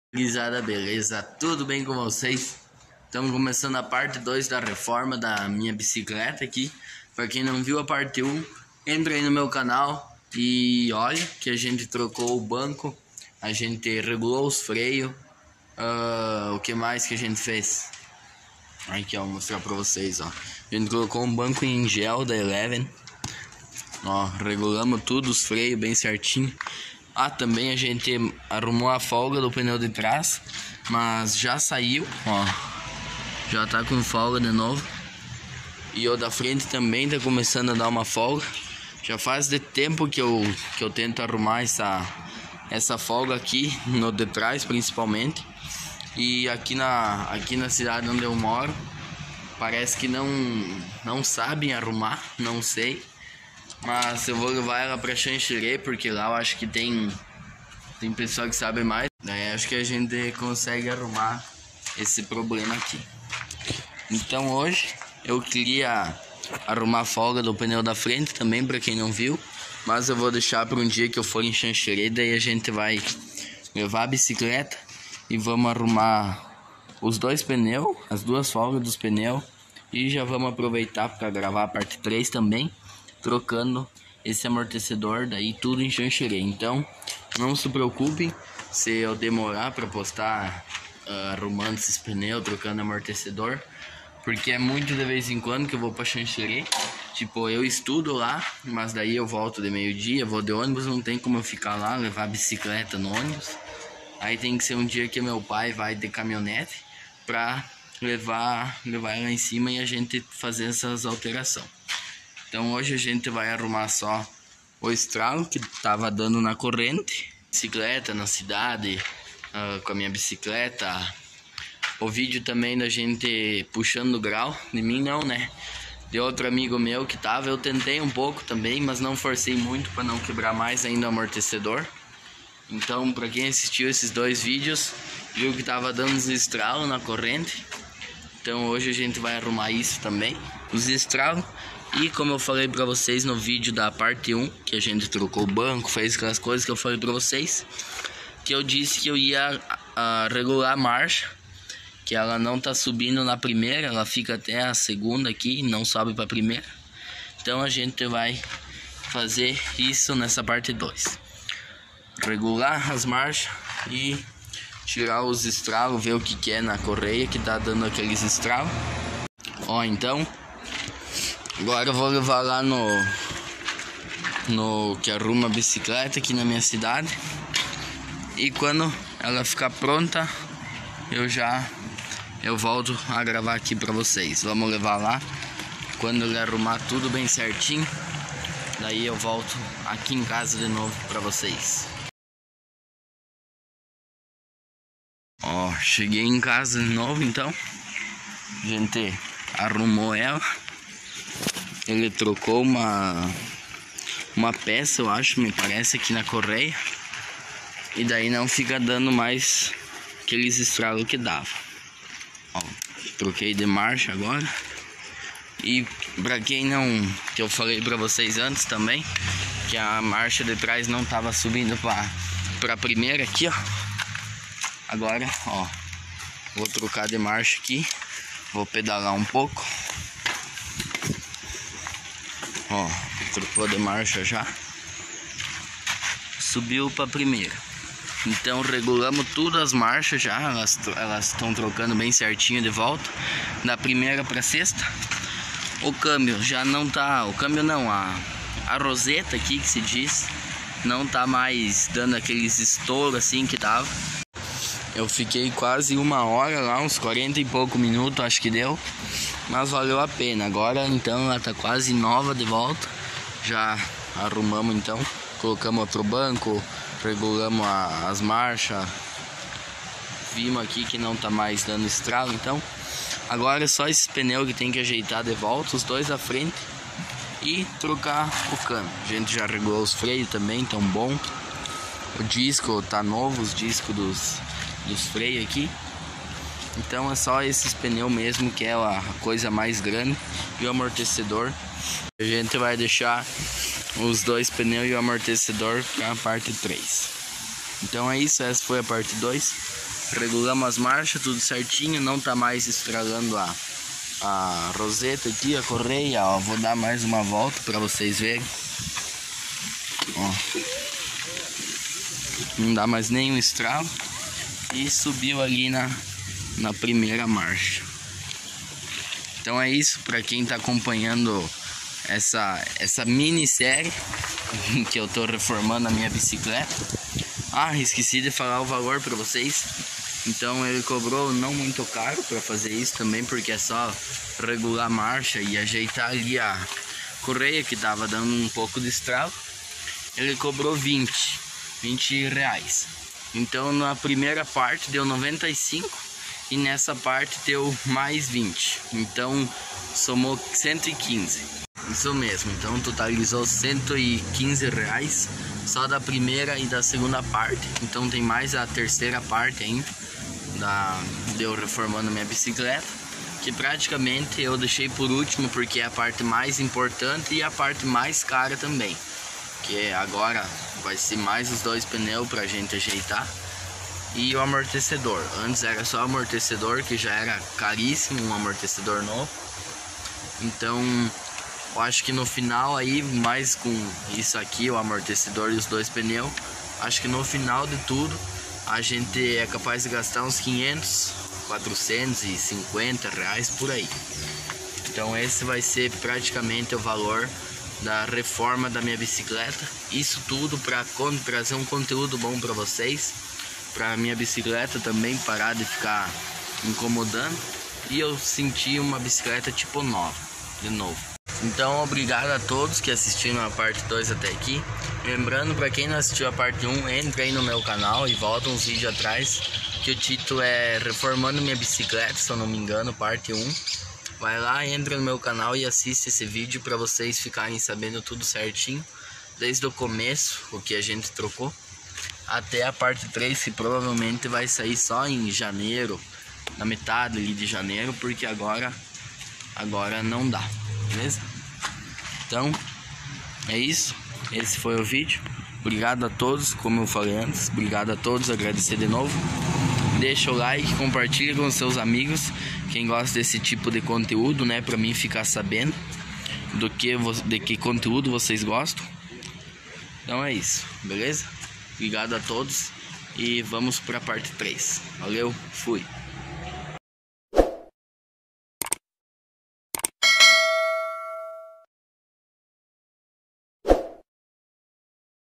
REFORMANDO MINHA BICICLETA (PARTE 2)
essa "mini série" é dividida em 3 partes, ent essa é a 2° parte, pra quem não viu a 1° parte, entra aí no meu canal e assiste desde o começo pra vcs ficarem sabendo de todas as trocas que a gente fez, obrigado!!